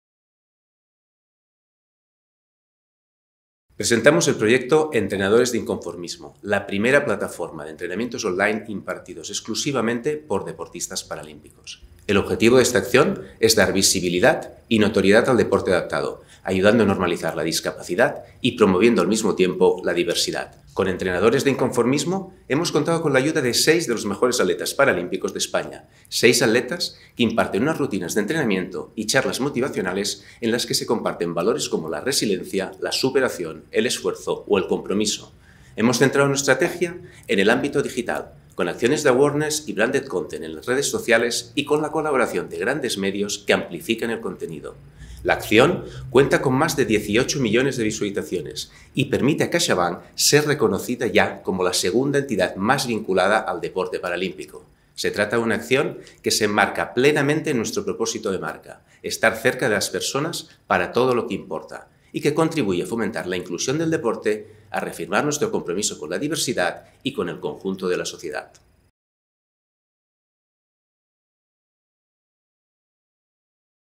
Categoría MARKETING SOCIAL: Caixabank | Finalista en la XV edición de los
Óscar López de Murillas, director de Marca de Caixabank, finalista en la categoría Marketing Social en la XV edición de los Premios Nacionales de Marketing, presenta su caso. ¡El 22 de junio conoceremos a los ganadores!